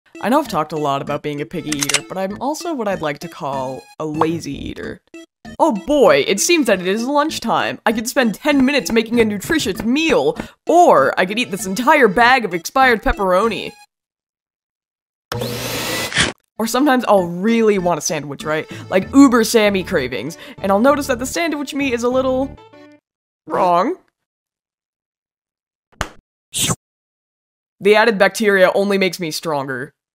I ate ROTTEN meat.
I eat rotten meat. Okay hear me out, I'm too lazy to go and get actual good food so I just settle for whatever (slightly deadly) stuff I find in my fridge. And nothing bad has happened to me yet, so I must be immune.